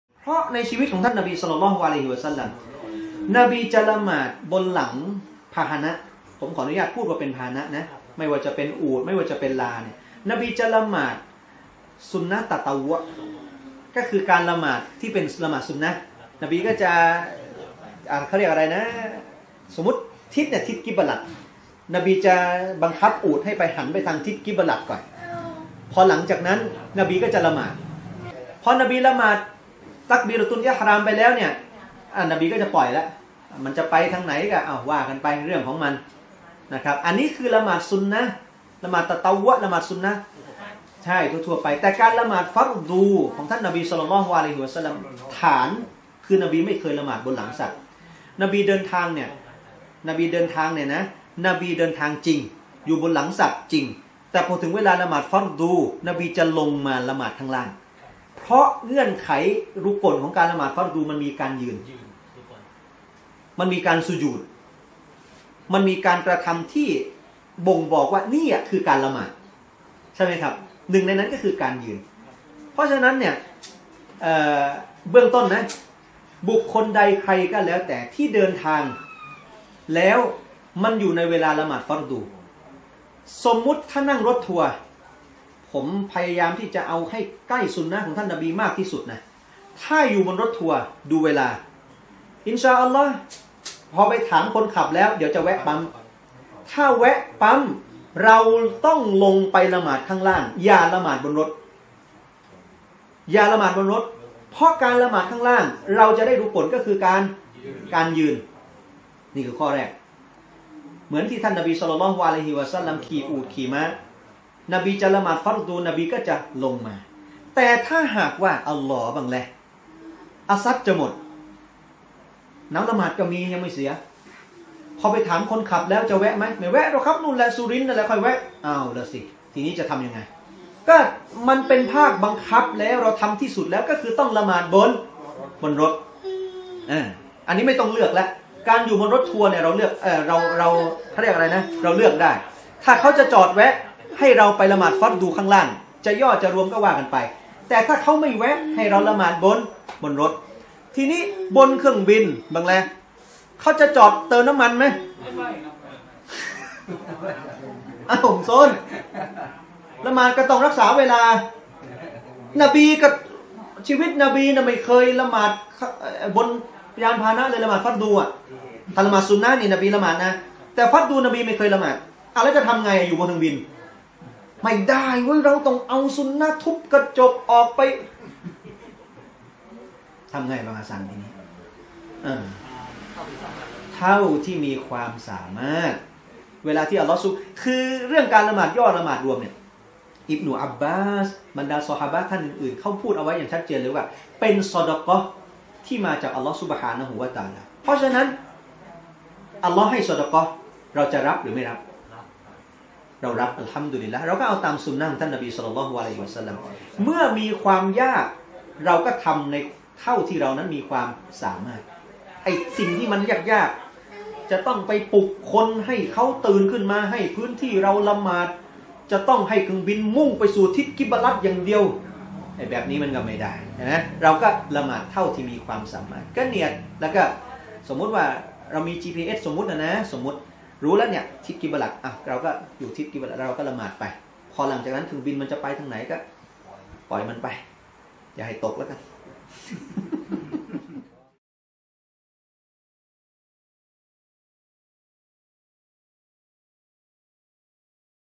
AH 2808 Q&A ละหมาดบนหลังสัตว์ หรือบนยานพาหนะได้หรือไม่ โดย อ.อับดุลฮากีม มังเดชะ
🎙 ตอบคำถาม โดย อ.อับดุลฮากีม มังเดชะ
⏰ 20.11.64 @ บ้านฮัจญีซอและห์ สว่างภพ ซ.ผดุงพันธ์ 23 หนองจอก กทม.
Tel & Line iD :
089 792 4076 (ดาวุด)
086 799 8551 (มูฮัมหมัด)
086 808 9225 (อับดุลการีม)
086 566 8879 (อนัส)